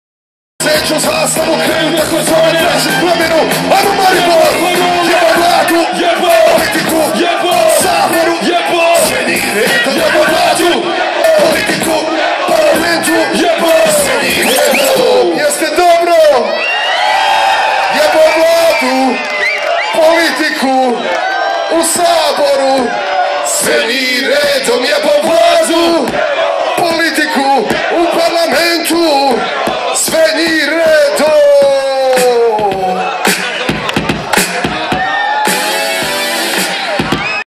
EDO MAAJKA - Festival Lent 2013 (jebo vladu)
Edo Maajka - Nastop na večerovem odru na Festivalu Lent 2013 v Mariboru